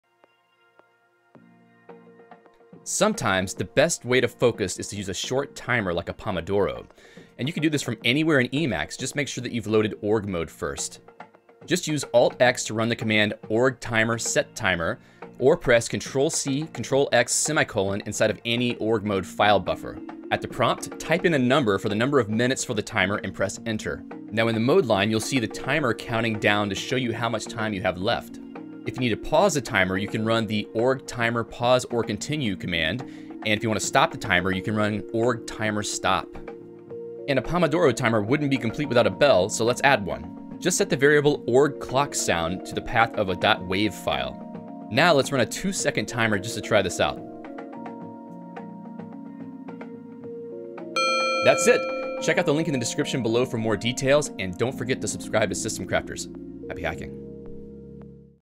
Emacs Has a Built-in Pomodoro Timer??
In this video, I'll show you how you can use the 'org-timer-set-timer' command in Emacs to emulate a Pomodoro timer for increased productivity.  No third-party packages needed, this is a built in feature of Emacs!